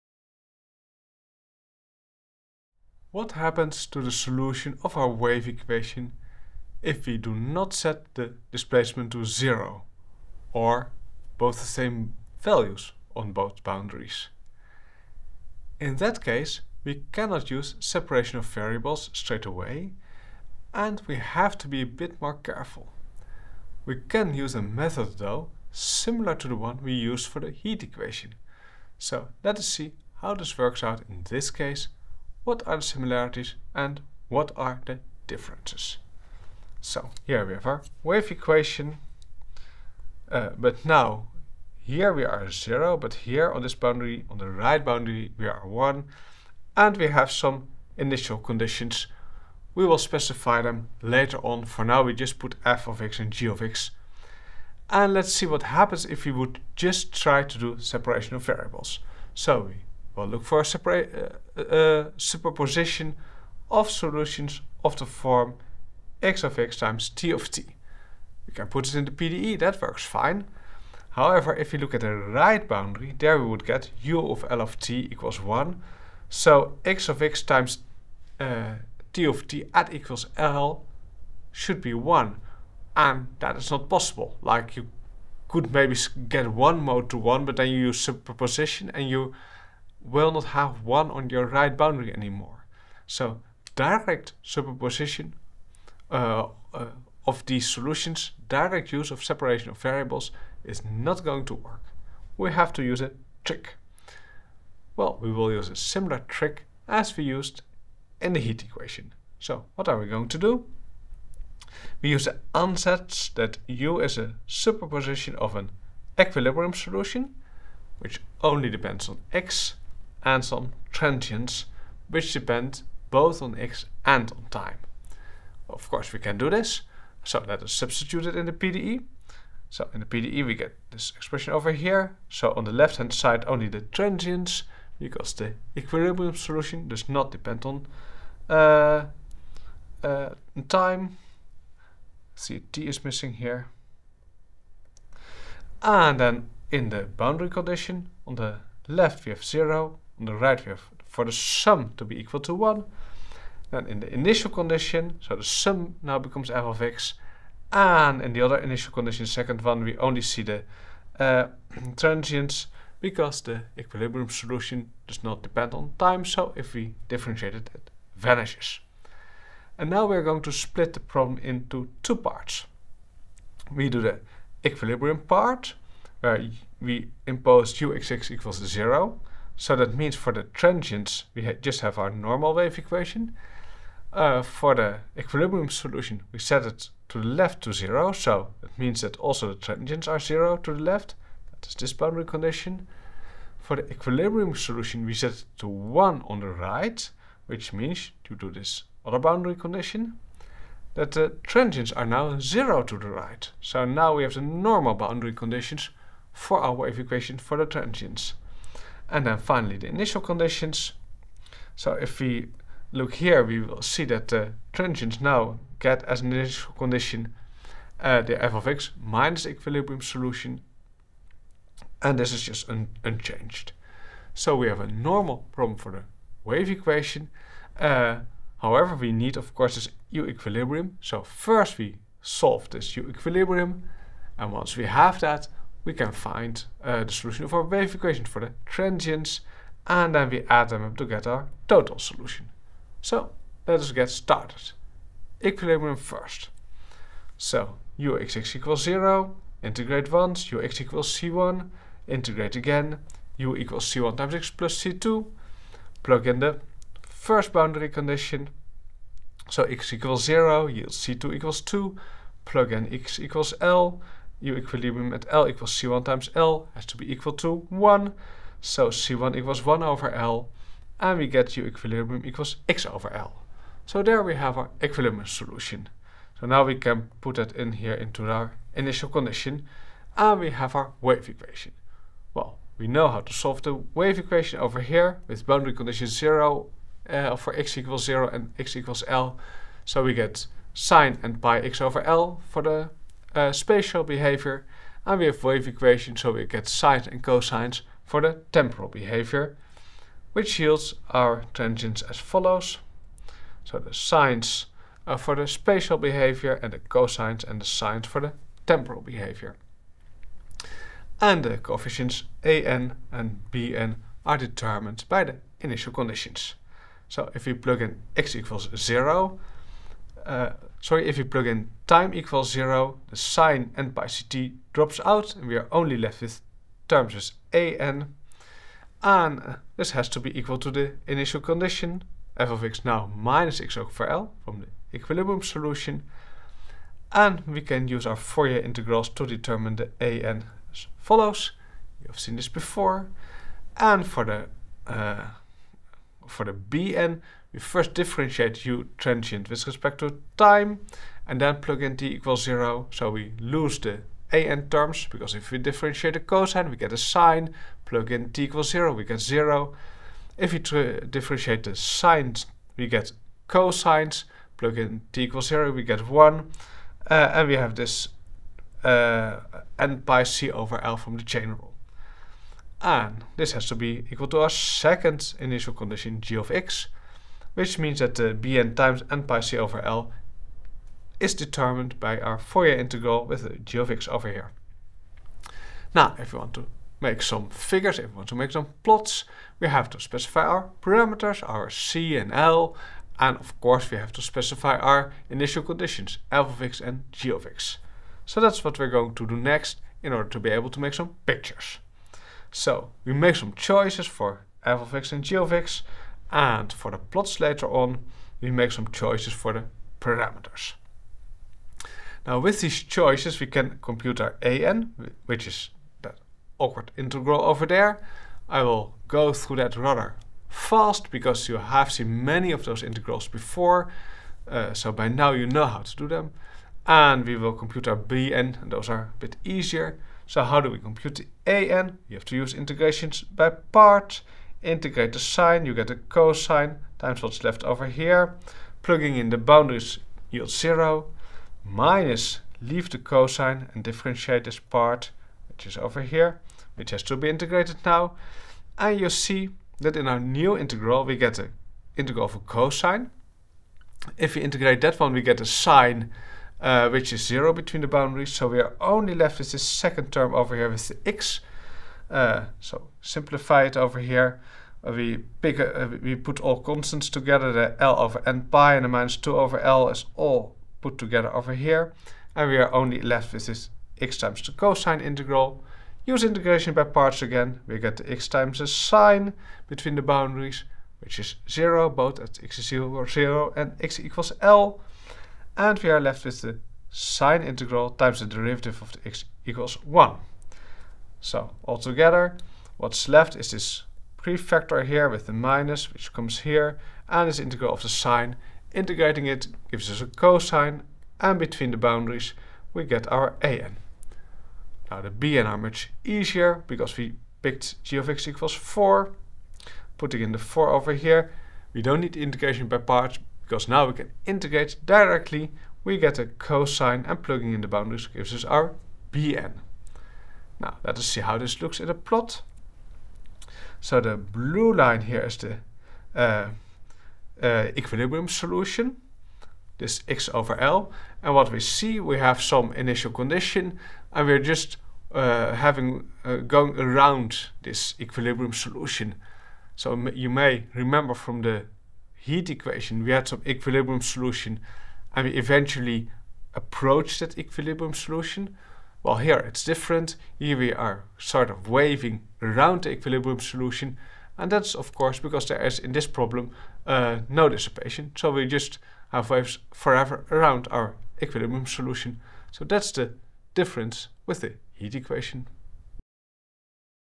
The wave equation - example two
What happens to the solution of our wave equation if we do not set the displacement to zero - or the same values - on both boundaries?
In that case we cannot use straight away and we have to be a bit more careful.
We can use a method though similar to the one we used for the heat equation. So let us see how this works out in this case - what are the similarities, what are the differences?